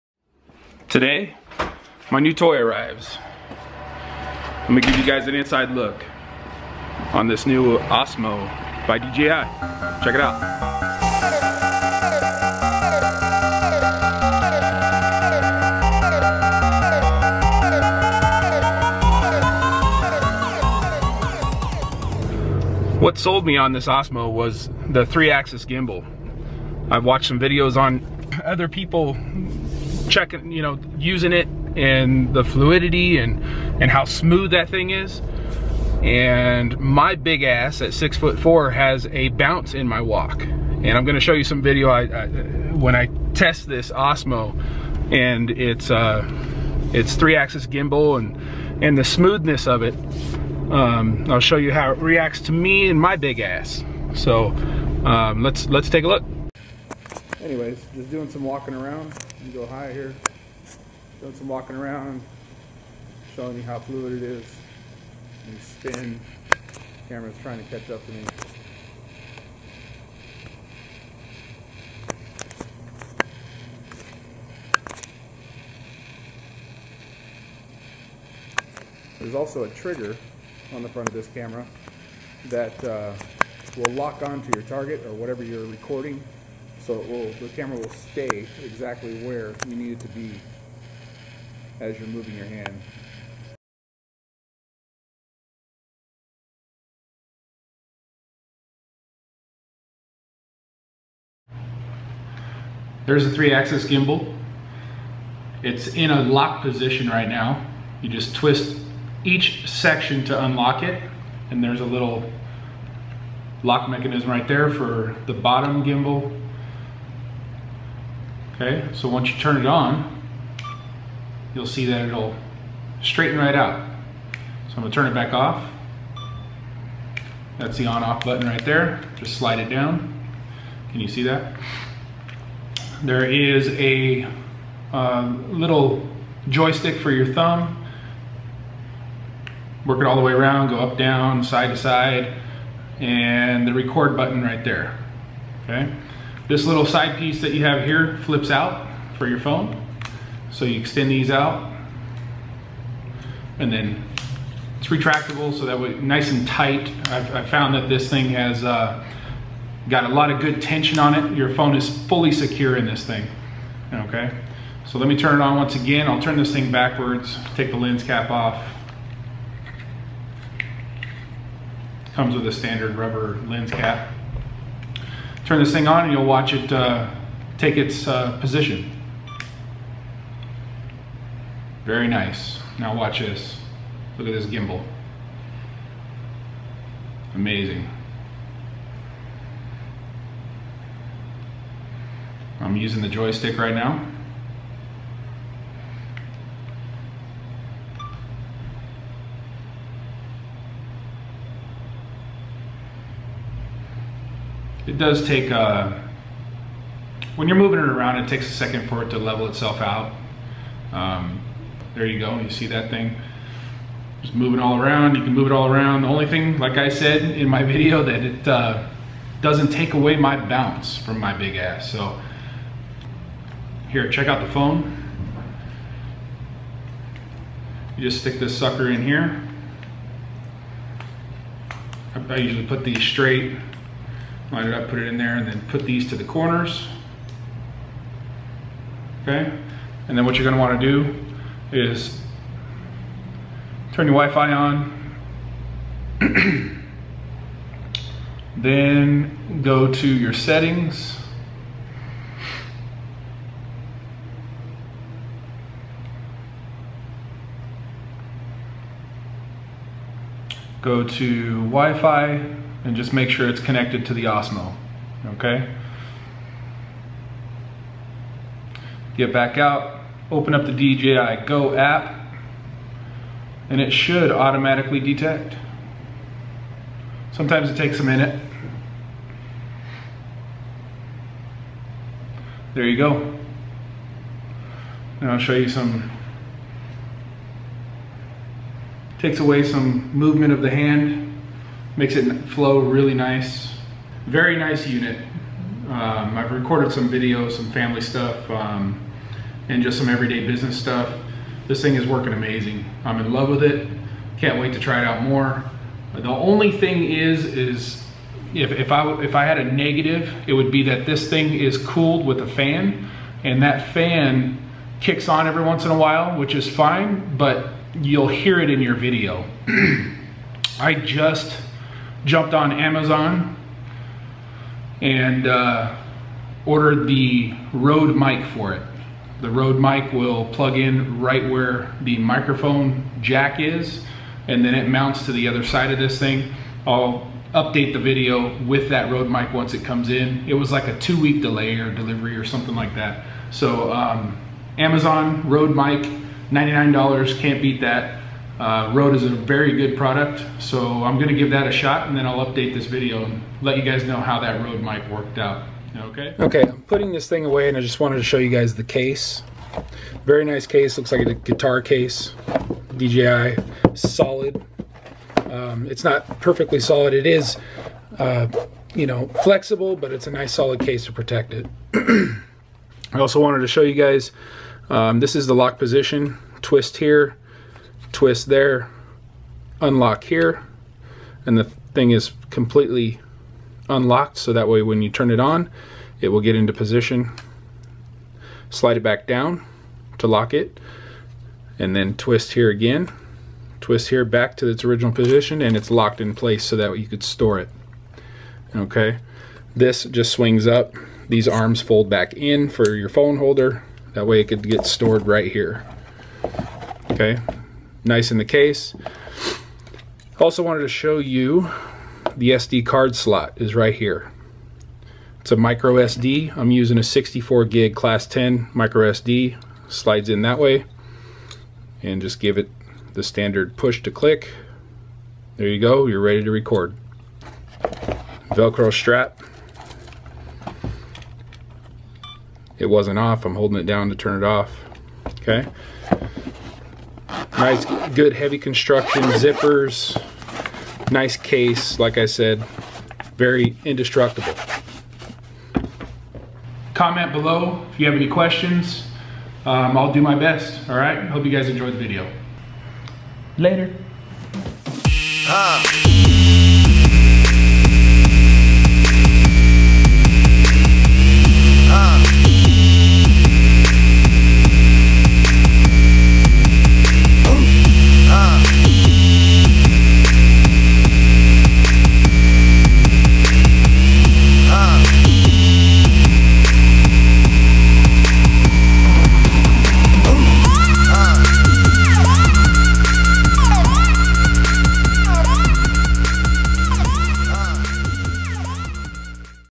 DJI Osmo Unboxing and Review 4K
I am not a f*ucking professional and this is just my personal opinion. It's my first unboxing and review so go easy on me!